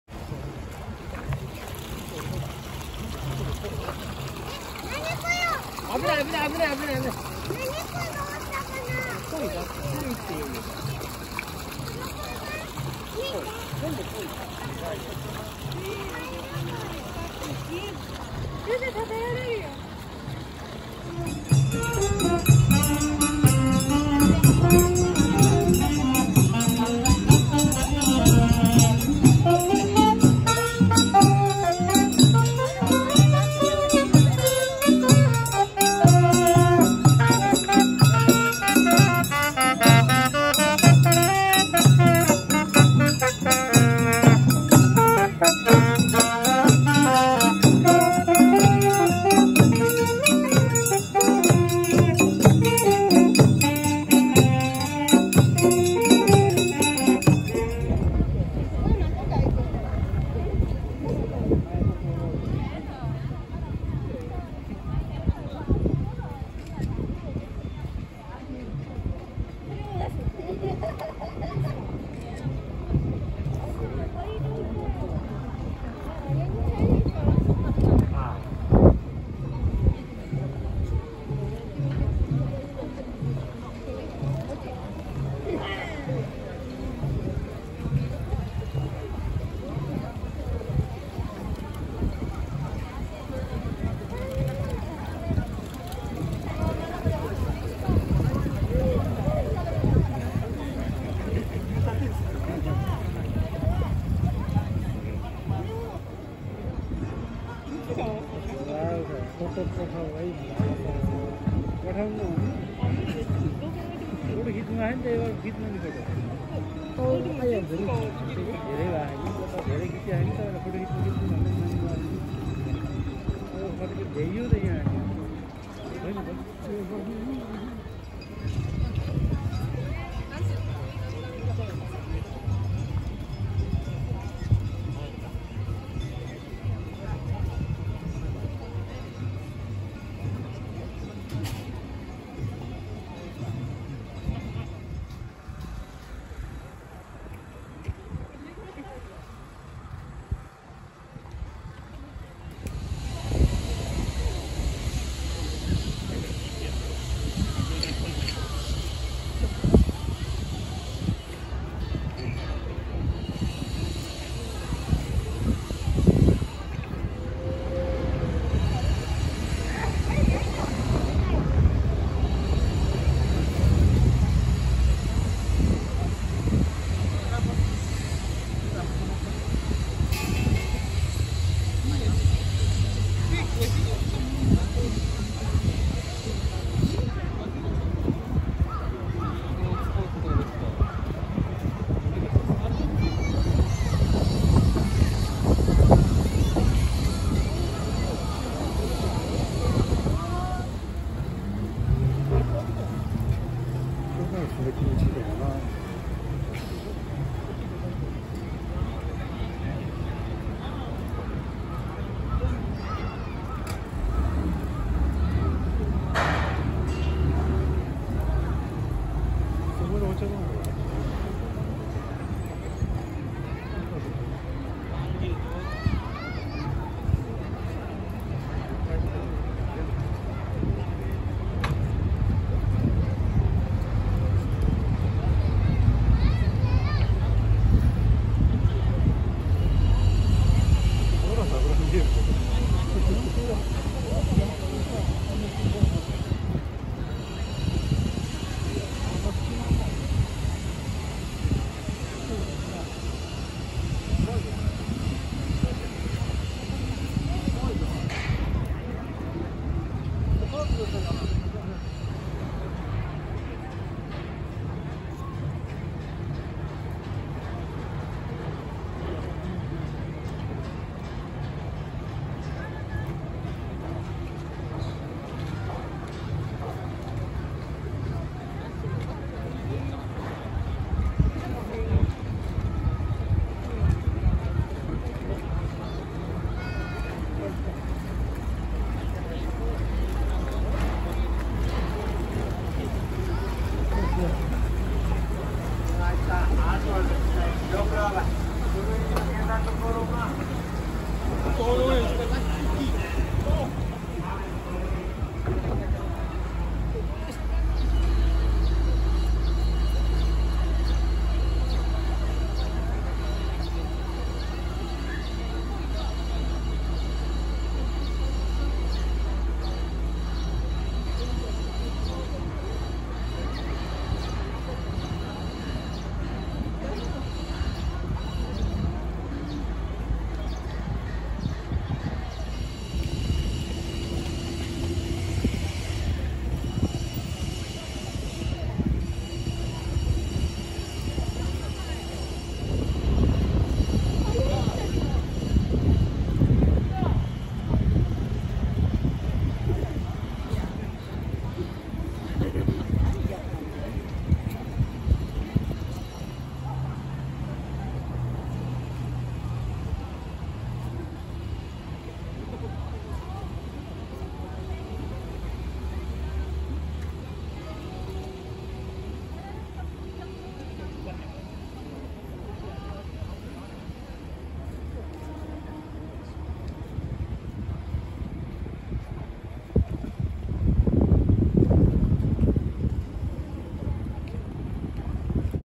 越中富山 富山城と松川公園 Cherry Blossoms in Toyama Castle.
2022年4月9日撮影。富山城、松川公園の桜です。初めてチンドン屋を見ました（笑）

Taken on April 9, 2022.  It is the cherry blossoms of Toyama Castle and Matsukawa Park.